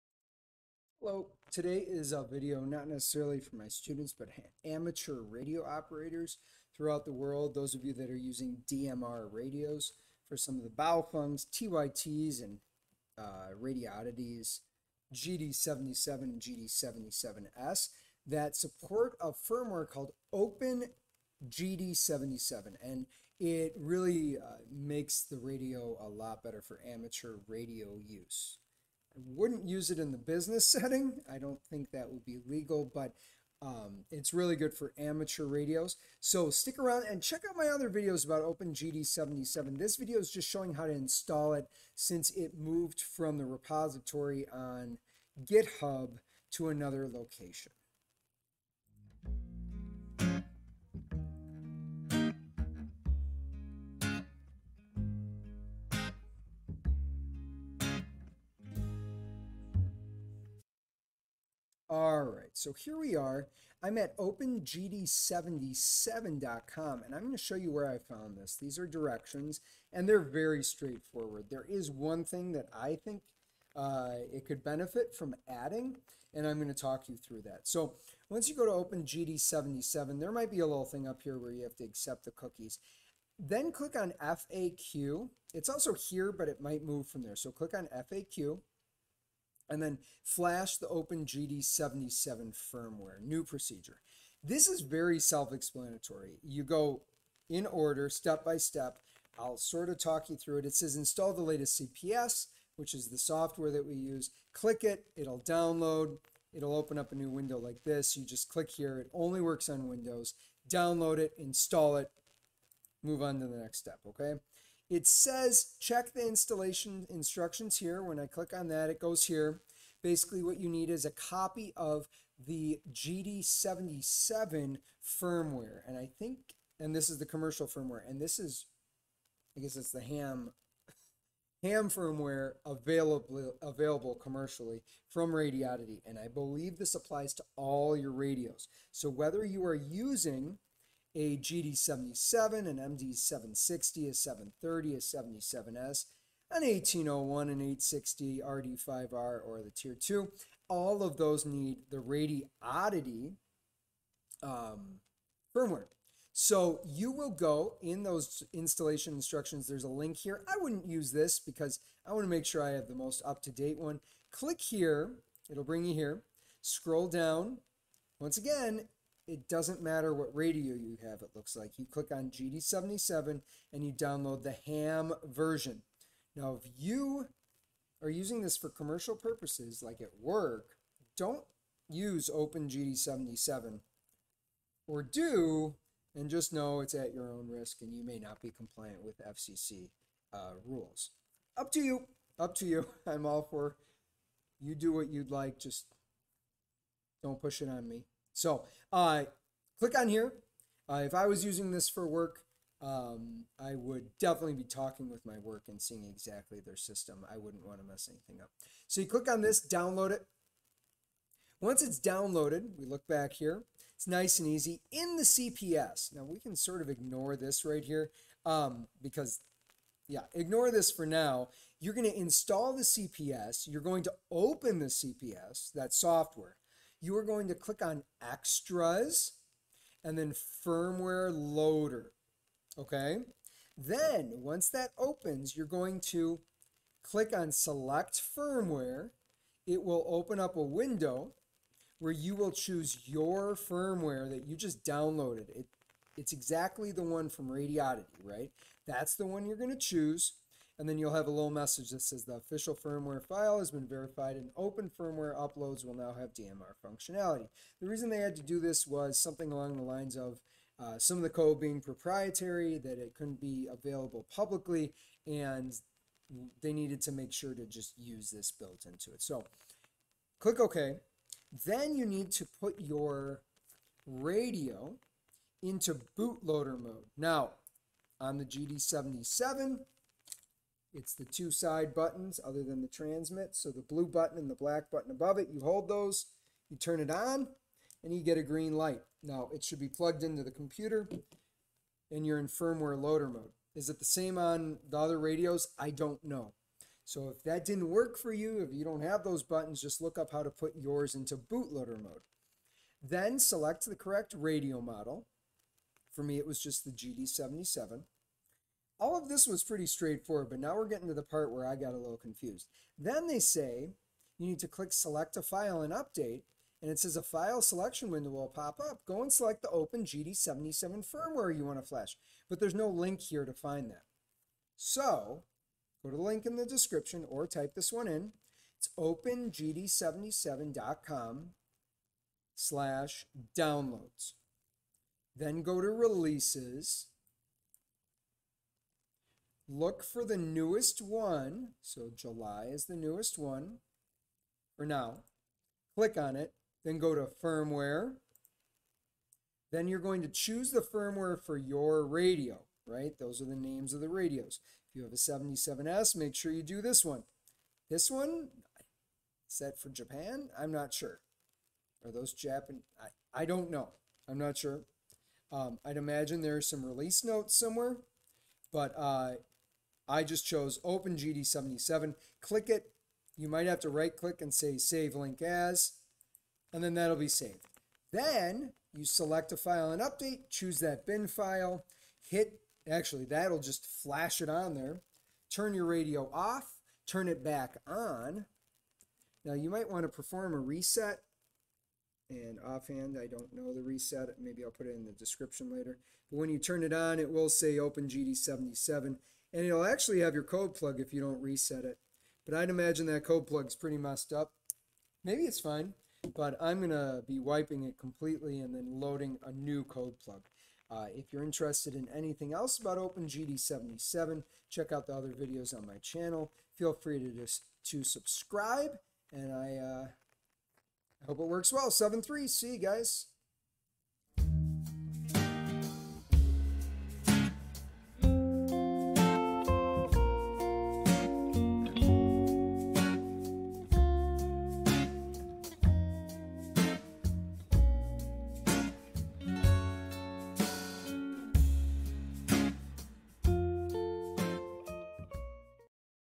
How do I install OpenGD77? Where are the files? GD77, GD77s, MD-760, MD-730, DM-1801, DM-860, RD-5R
With OpenGD77 not being on Github anymore, there is a new place to find the files and install the latest version of OpenGD77.  If you'd like to improve your GD77, GD77s, MD-760, MD-730, DM-1801, DM-860, or RD-5R you should check it out!